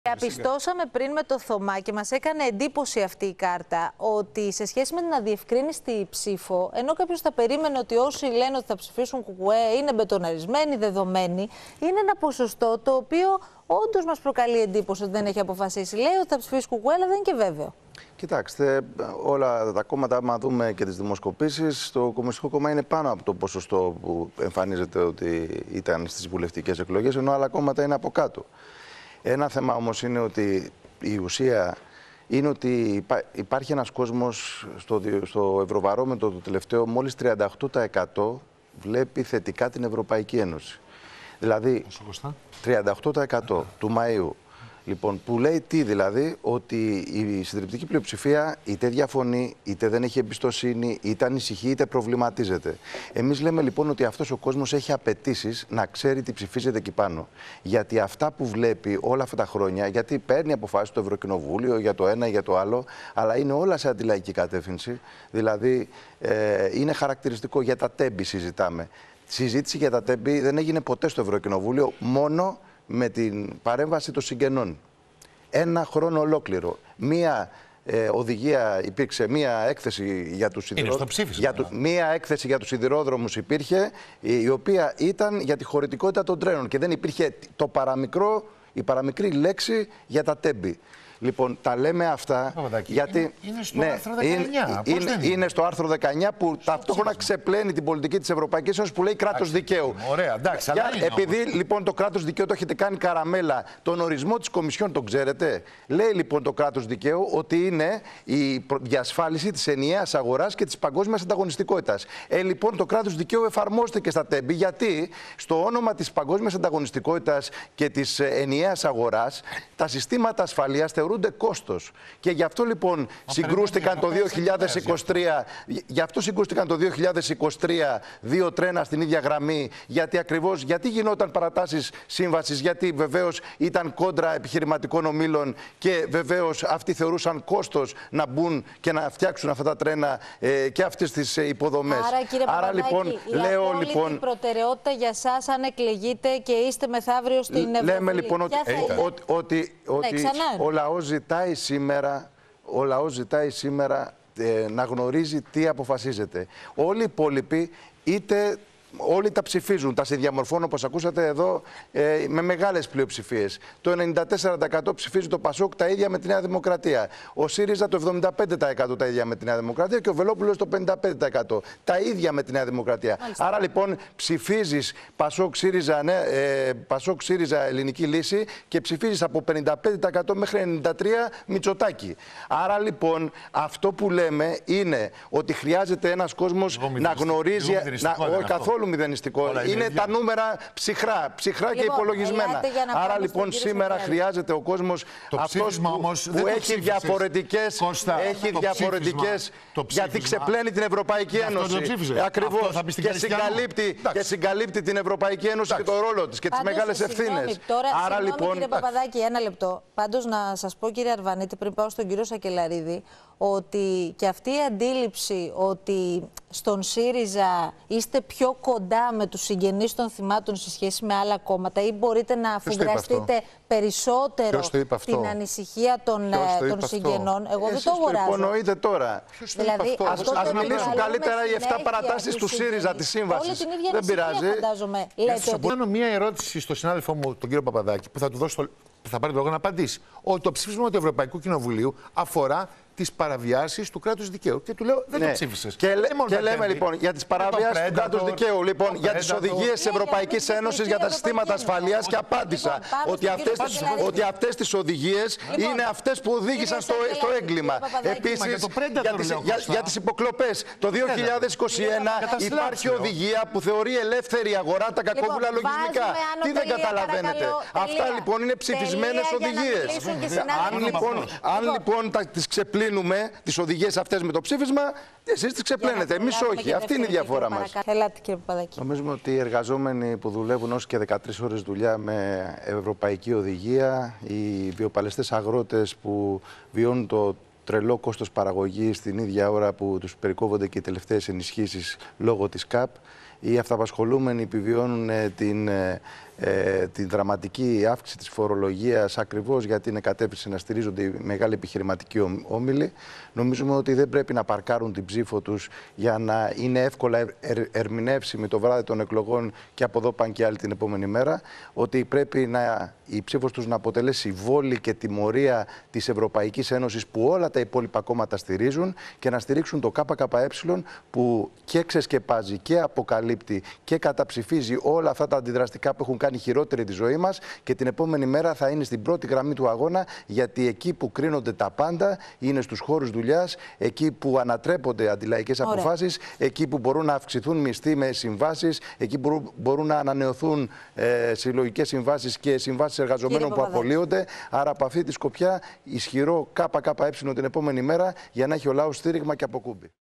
Κ. Παπαδάκης: Ισχυρό ΚΚΕ για να έχει ο λαός αποκούμπι την επόμενη μέρα
Οι αποφάσεις που λαμβάνονται στο Ευρωκοινοβούλιο είναι όλες σε αντιλαϊκή κατεύθυνση, τόνισε ο *Κώστας Παπαδάκης,* ευρωβουλευτής του ΚΚΕ και εκ νέου υποψήφιος μιλώντας σε εκπομπή της τηλεόρασης του Ant1, για τις ευρωεκλογές.